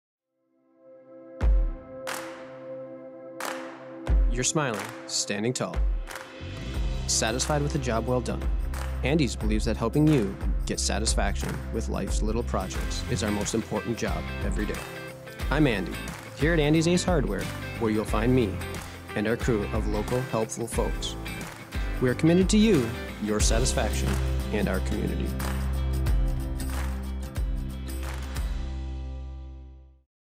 Andy's Ace Hardware - Shop Local
You're smiling, standing tall. Satisfied with a job well done. Taking pride in a job done right starts with the right tools. At Andy's, helping you get satisfaction from life's little projects is our most important job. At Andy's Ace Hardware, our crew of local, helpful folks is committed to you, your satisfaction, and our community! Shop local at Andy's Ace Hardware today.